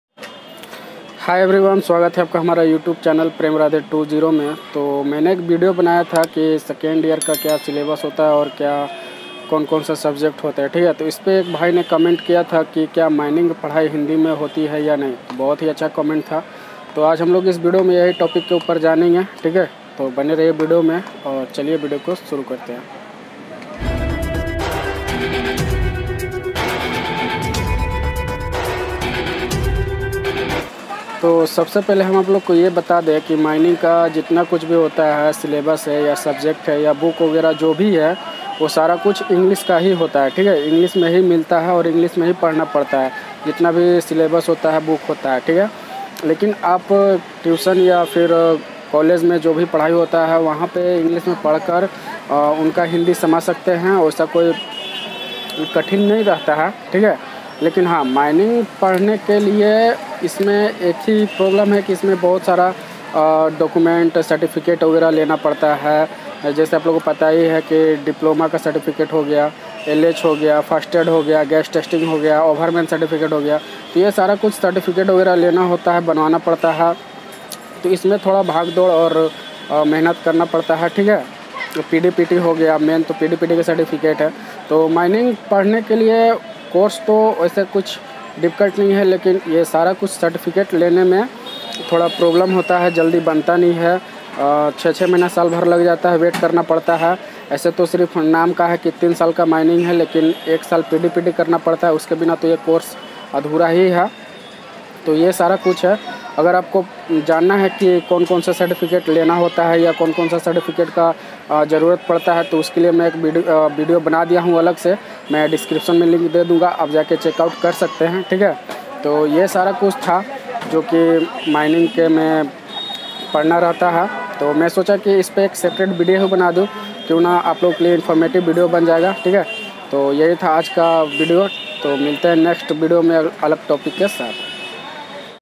Diploma In Mining Engineering || क्या माइनिंग के पढ़ाई हिन्दी में होती है या नहीं
Diploma In Mining Engineering || क्या माइनिंग के पढ़ाई हिन्दी में होती है या नहीं ? || कोर्स के पुरी जानकारी !

Your query:
Diploma in mining engineering

From Palamu Jharkhand.